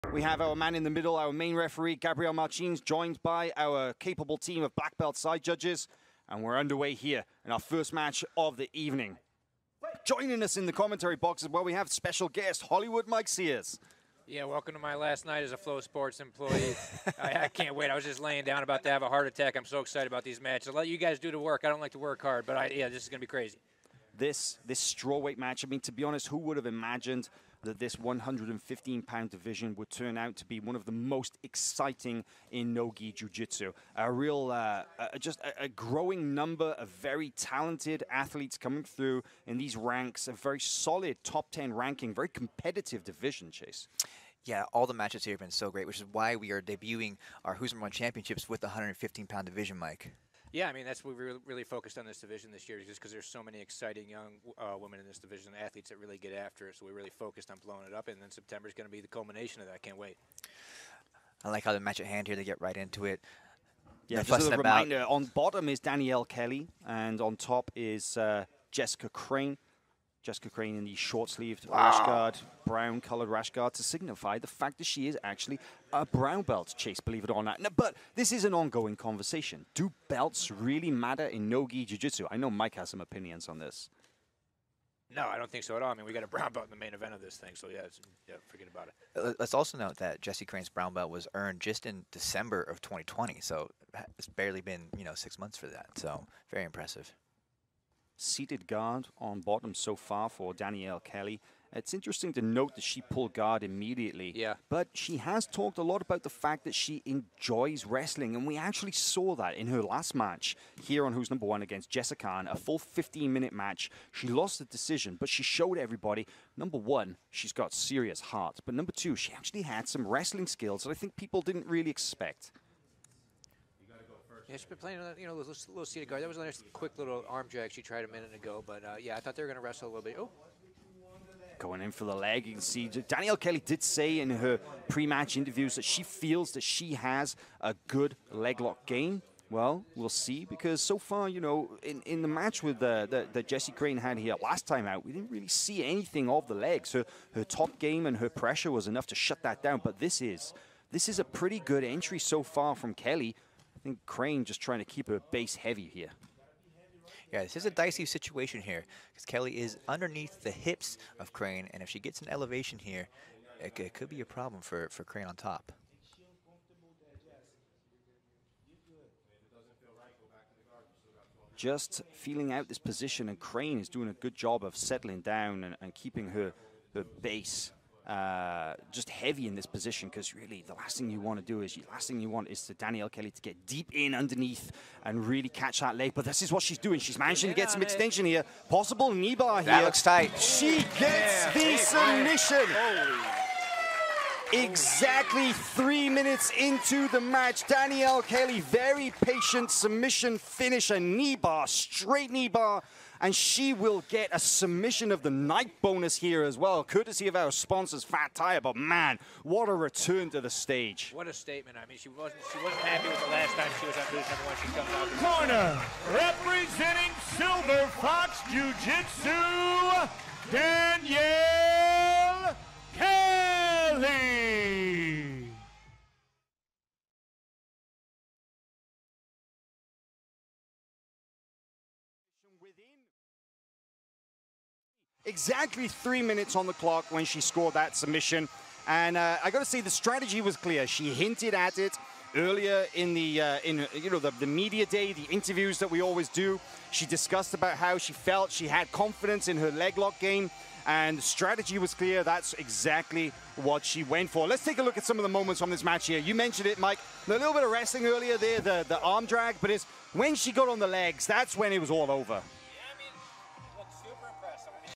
Underdog Danielle Kelly submits opponent kneebar in 3 minutes
This video is owned and by Flograppling